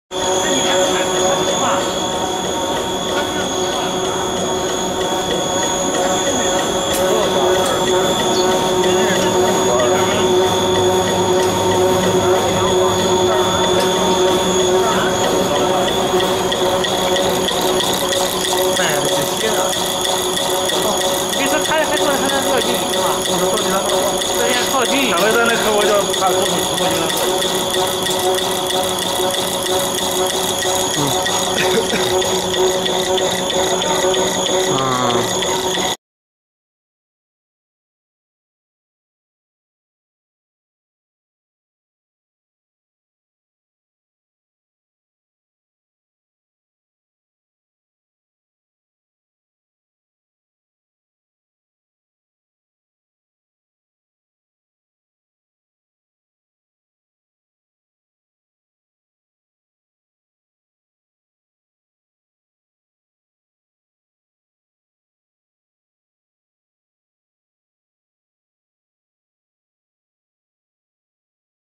macaroni machinery
macaroni processing line include mixer-hoister-extruder-dryer-cooling